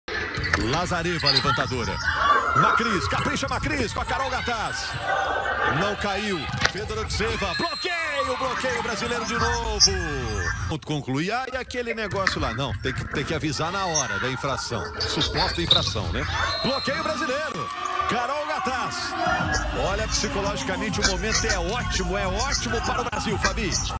Carol Gattaz faz dois bloqueios no 1º set - BRA x RUS (Liga das Nações 2021)
Siga o Block Brasileiro no Twitter
A central Carol Gattaz vem crescendo no fundamento bloqueio nesta Liga das Nações.

Imagens: SporTV/FIVB/Reprodução
Foto: FIVB/Divulgação